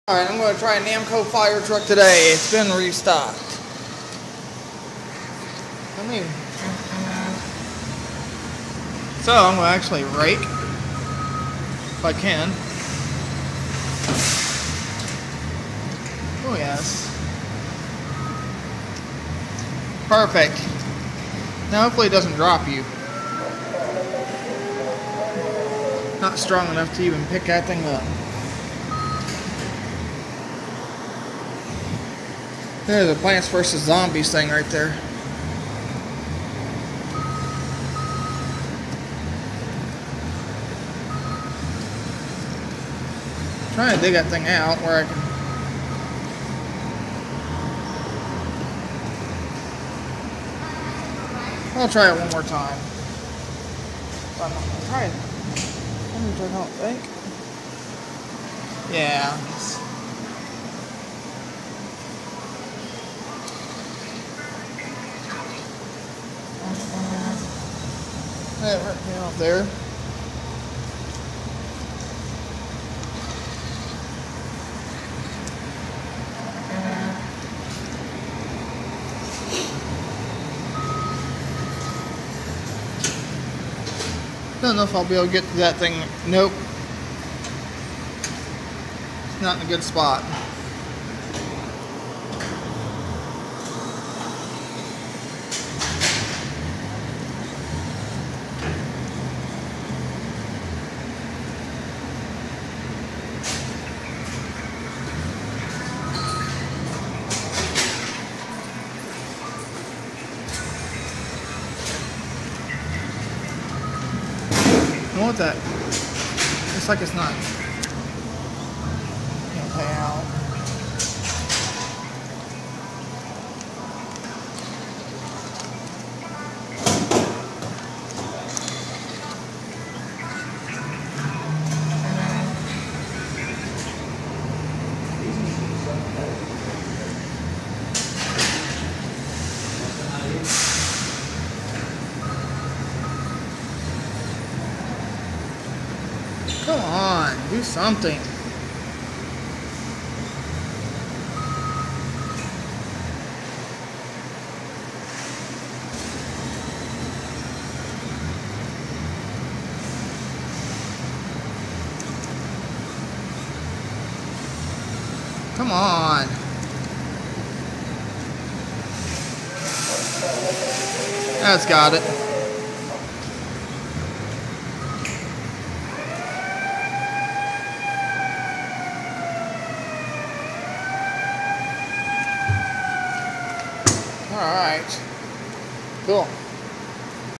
NAMCO FIRE TRUCK CLAW MACHINE @WALMART TOUGHER TO WIN
THIS WAS FILMED ON FRIDAY AT MY LOCAL WALMART HERE I AM PLAYING THE NAMCO FIRE TRUCK CLAW MACHINE   HERE I AM  FIGHTING A TOUGH PLAY FIELD AFTER NAMCO RESTOCKE THE CLAW